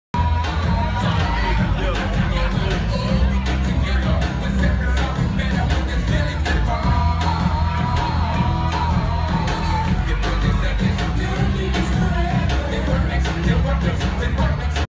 Panthers quarterback Cam Newton is dishing out Thanksgiving cheer...
Panthers quarterback Cam Newton is dishing out Thanksgiving cheer instead of dabbin' at his annual Thanksgiving Cam Jam for more than 400 local kids. Video by David Newton